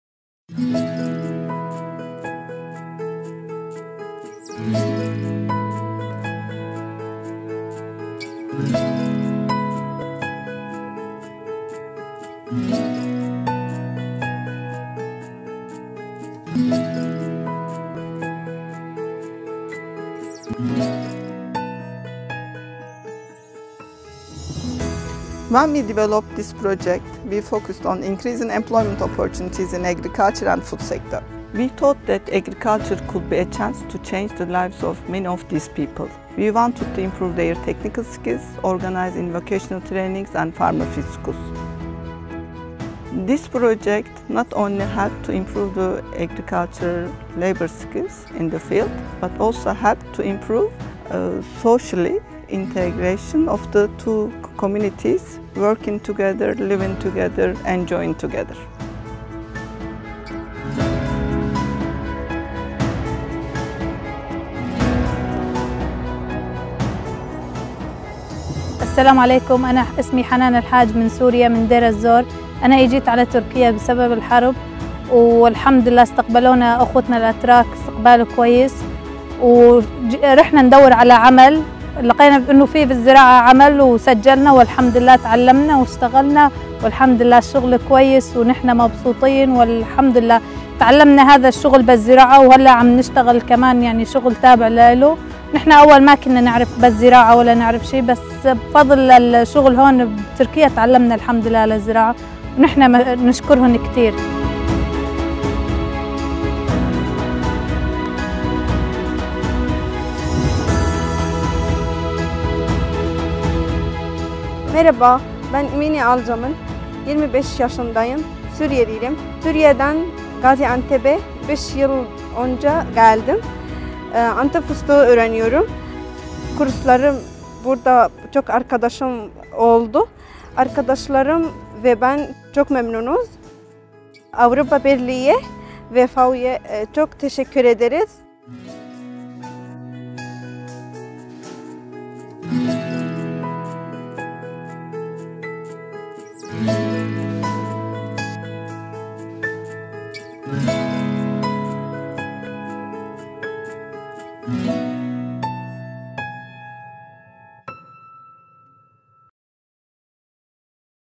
FAO Türkiye : Güçlü Geçim Dayanıklı Toplum Projesi
Birleşmiş Milletler Gıda ve Tarım Örgütü’nün Avrupa Birliği finansmanı ile hayata geçirdiği, T.C. Tarım ve Orman Bakanlığı ve T.C. Çalışma ve Sosyal Güvenlik Bakanlığı ile ortaklaşa yürütülen projede, sosyo-ekonomik entegrasyonun desteklenmesi ve geçim fırsatlarının yaratılması yoluyla Türkiye'deki Geçici Koruma altındaki Suriyelilerin ve Ev Sahibi Toplulukların dayanıklılığının arttırılması amaçlanmaktadır.